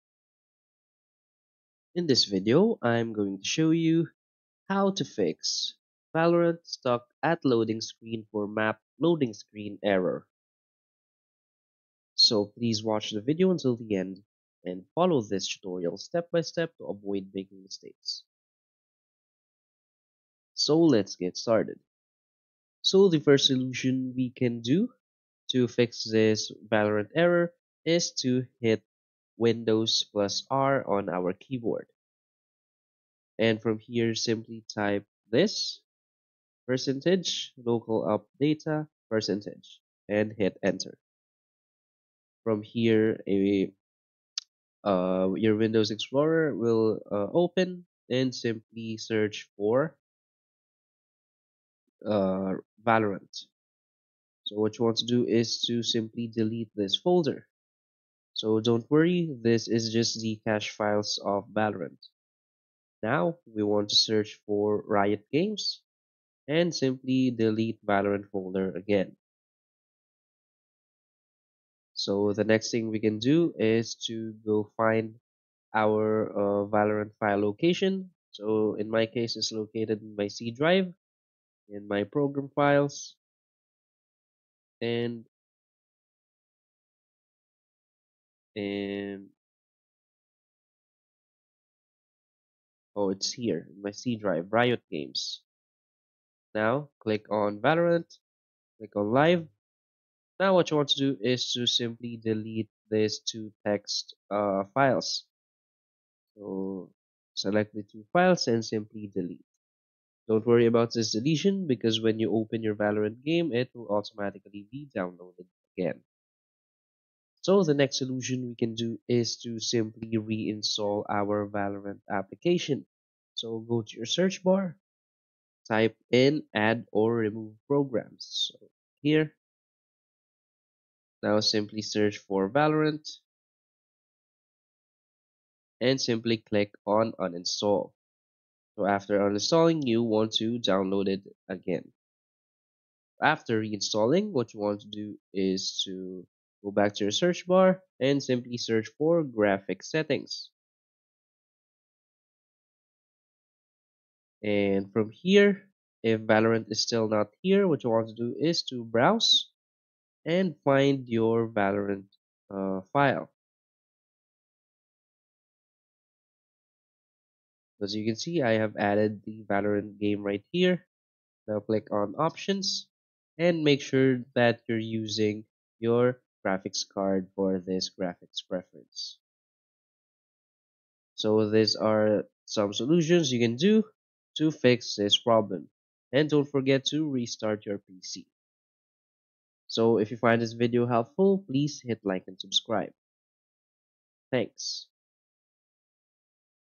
Fix Valorant Stuck at Loading Screen for Map Loading Screen (2024)
In this video I show you how you can solve Valorant Stuck at Loading Screen. Do you wonder how you can resolve Valorant Stuck at Loading Screen? I'll show you exactly how to do that.